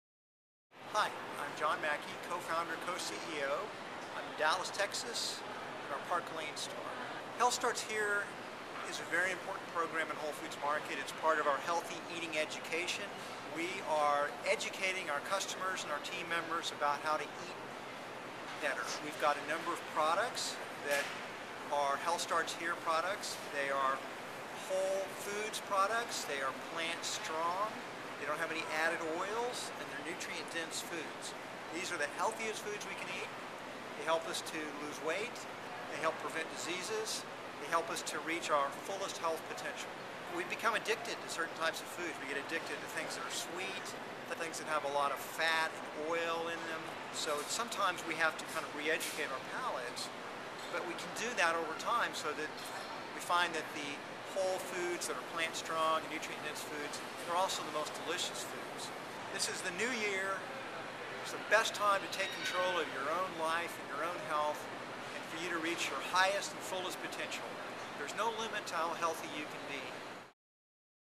John Mackey "Health Starts Here" Dallas
John Mackey tells why "Health Starts Here" in the aisles of Whole Foods Market in Dallas - January 2011